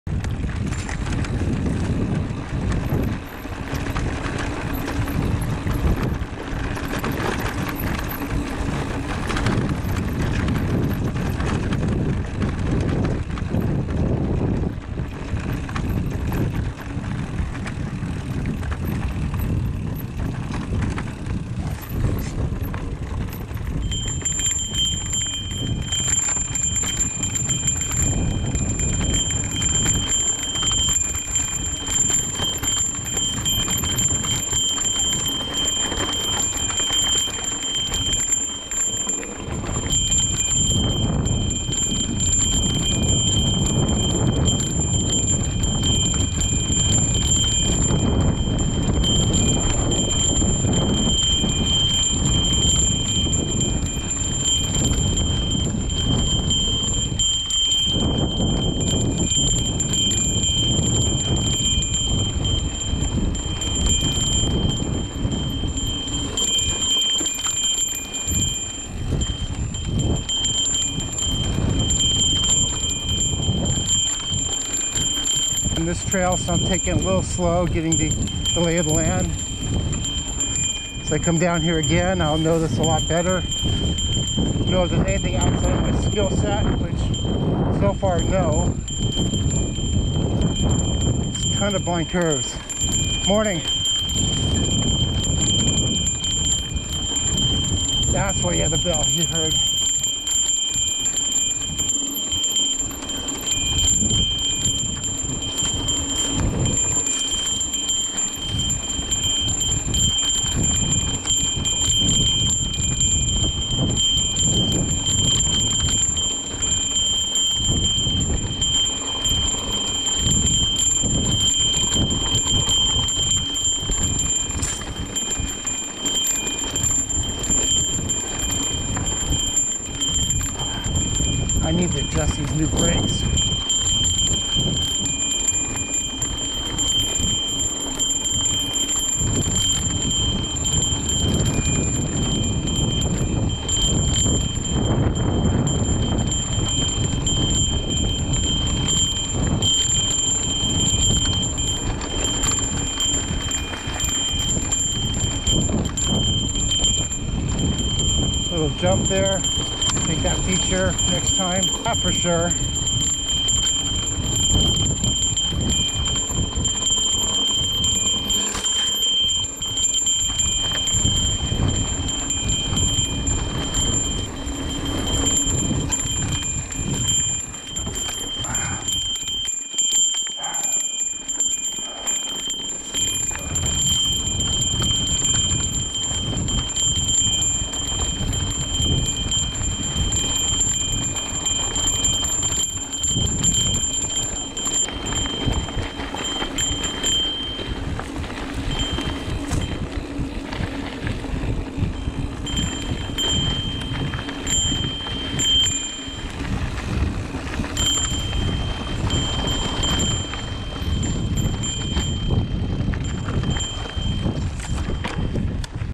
Chivo Canyon
First time mountain biking down Chivo Canyon